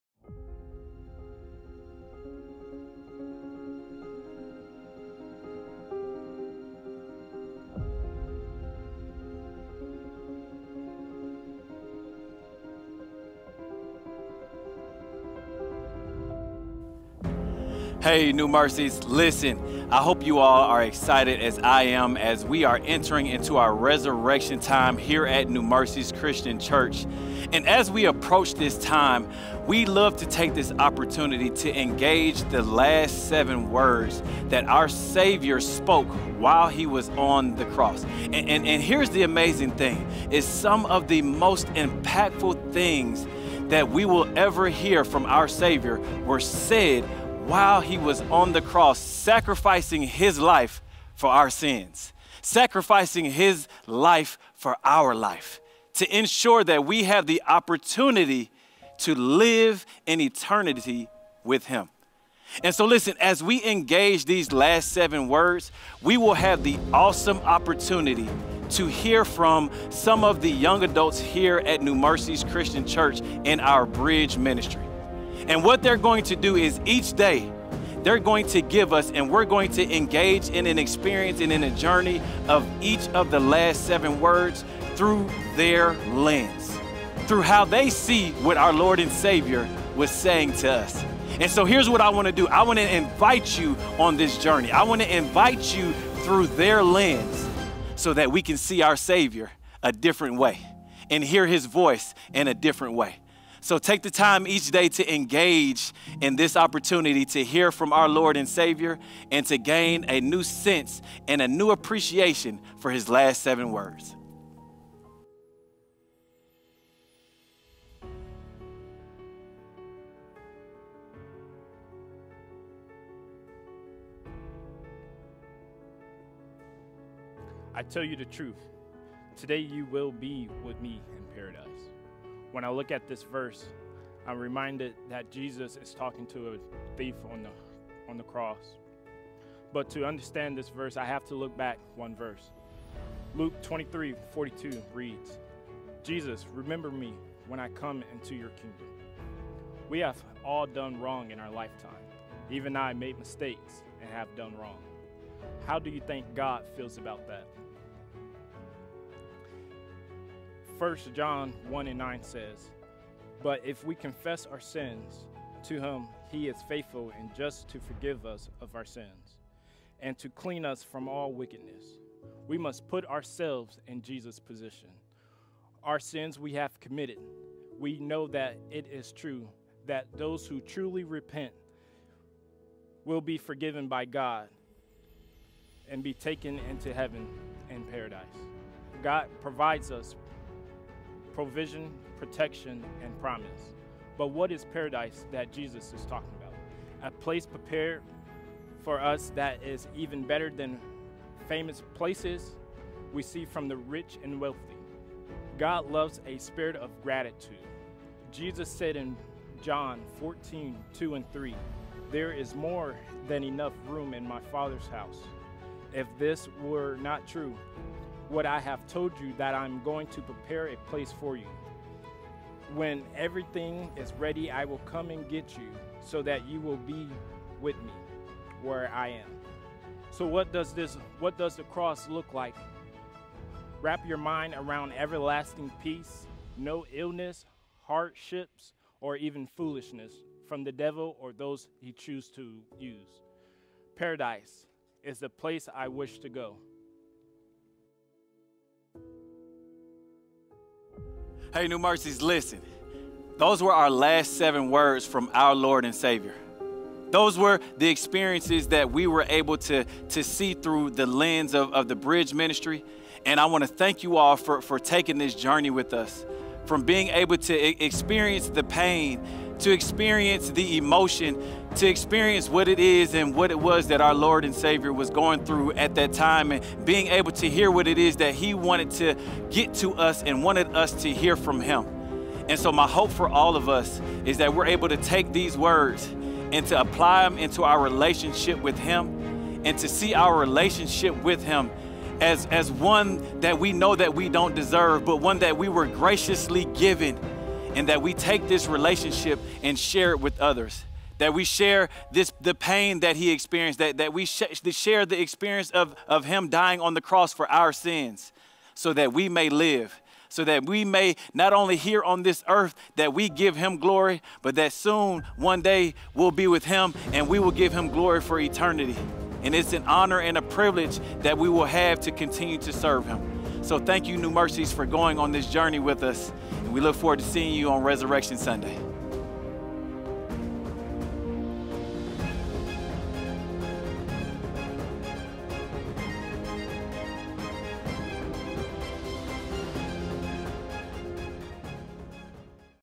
2nd Word of Christ
Significance of Jesus' Last Words - 2nd Word

This passage is a moving account of Jesus’ dying words. When everything was said and done, Jesus’ work on the cross was all but complete, and his proclamation, “Father, into your hands I commit my spirit!” finished the work. The significance of Jesus’ statement lies in a conversation he had with religious leaders about his role in God’s great plan:

I am the good shepherd. I know my own and my own know me, just as the Father knows me and I know the Father; and I lay down my life for the sheep. And I have other sheep that are not of this fold. I must bring them also, and they will listen to my voice. So there will be one flock, one shepherd.

For this reason, the Father loves me, because I lay down my life that I make take it up again. No one takes it from me, but I lay it down of my own accord. I have the authority to lay it down, and I have authority to take it up again. This charge I have received from my Father. (John 10:14)

Verily I say unto thee, To day shalt thou be with me in paradise. Luke 23:43

For more information about this ministry or this and other events go to newmerciesnow.org.